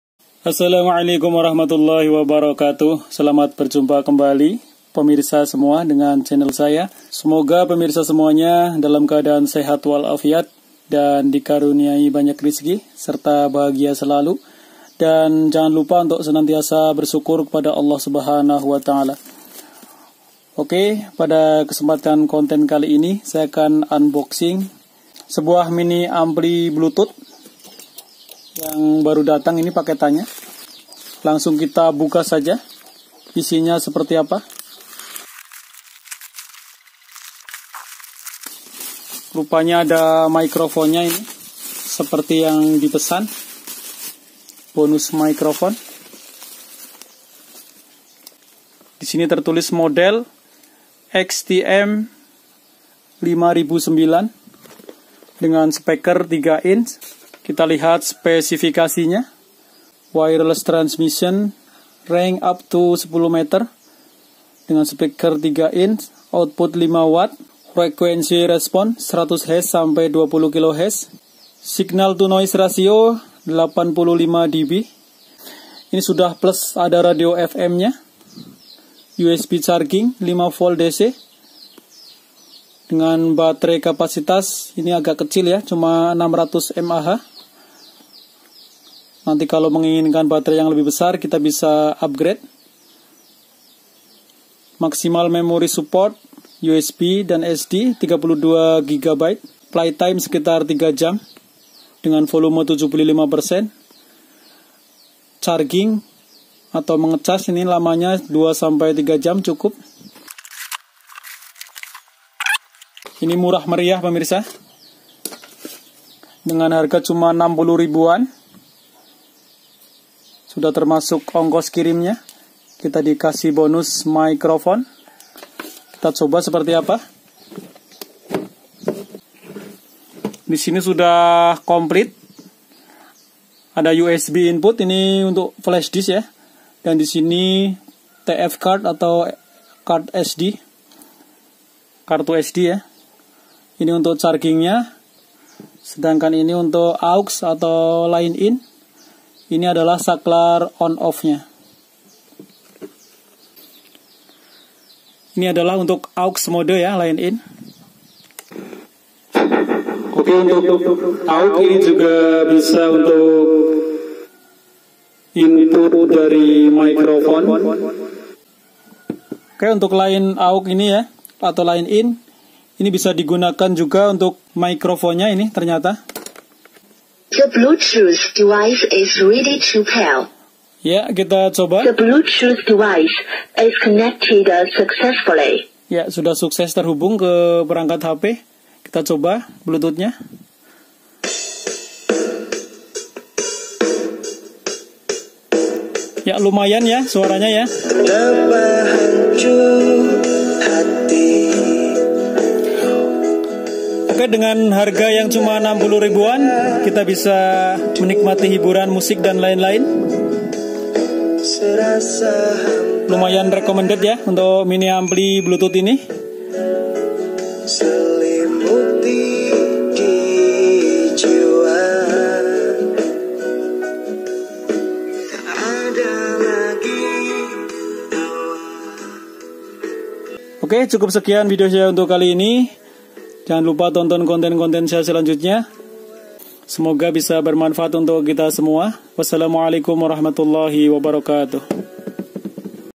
MINI AMPLI BLUETOOTH MURAH MERIAH RECOMMENDED
Unboxing dan Review mini ampli bluetooth,usb,fm, bonus microphone dgn harga murah meriah dan sangat Recommended.